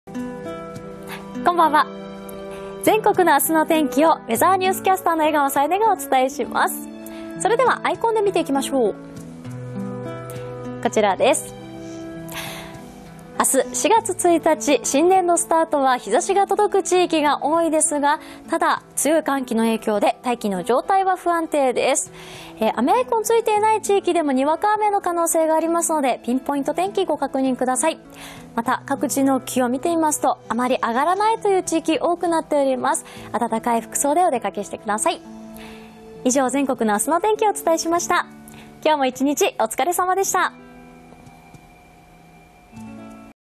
★お天気キャスター解説★ あす1日(土)の天気
明日4月1日(土)の天気を、ウェザーニュースキャスターの江川清音がお伝えします。
こんばんは。明日は、日差しが届く地域が多いものの、気温の上昇は鈍く、春は足踏み状態です。九州や東北太平洋側など、にわか雨の可能性があるので、折り畳み傘がお守りです！
いよいよ、新たしい年度がスタートします！時間にも心にも余裕を持って新年度を迎えましょう♪麗らかな気持ちでお過ごし下さい(*^^*)
今日も1日、お疲れ様でした。